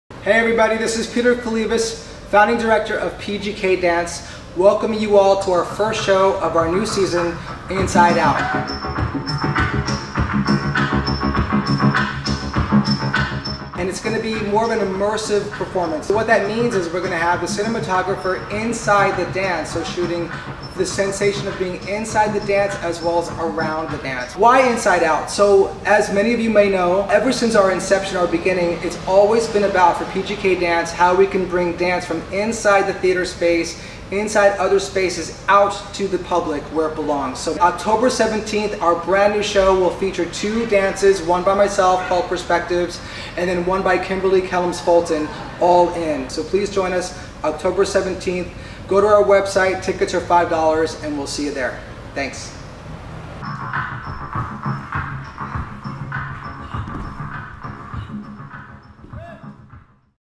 Peter welcome audience to Experience "INSIDE/OUT" immersive virtual Oct 17th @ 730pm PST
$5 Tix for ZOOM Access to produced World Premiere event